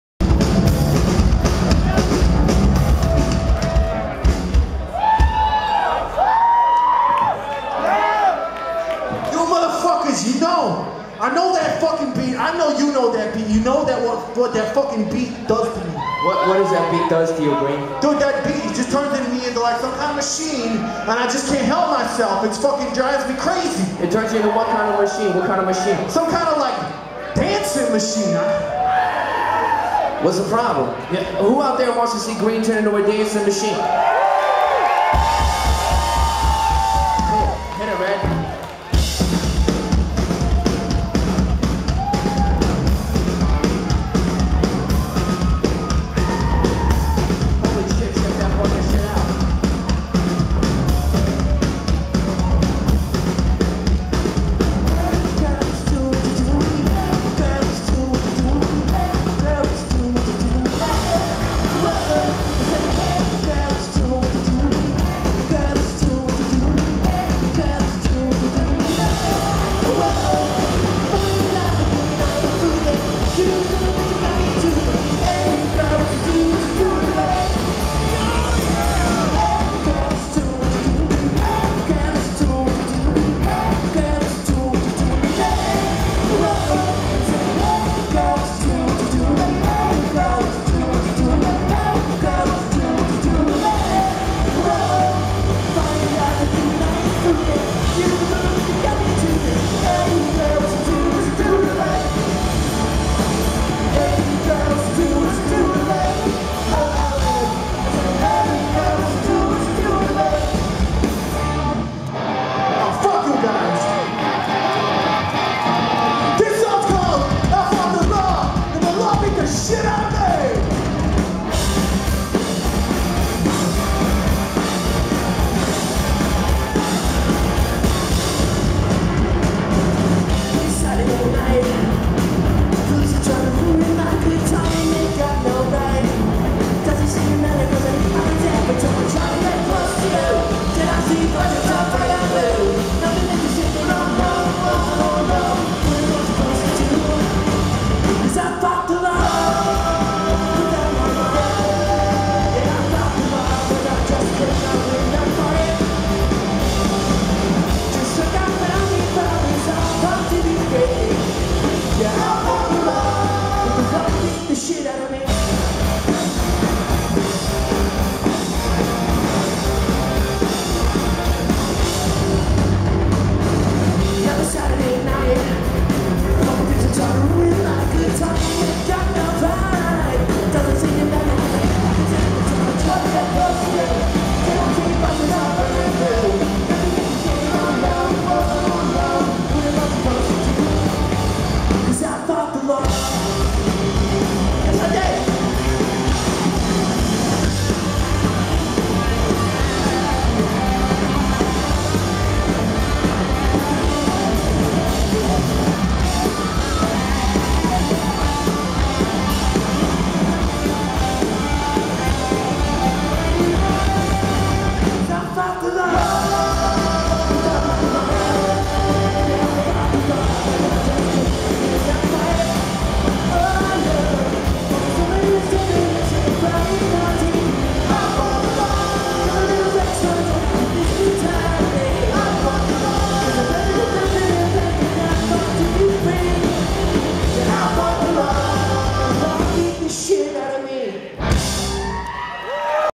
Masked Intruder @ Fest 12, IV
Masked Intruder, live @ Fest 12 (2013). FULL SET footage! HD Quality video w/ good sound. It'll take about a week to upload all of them.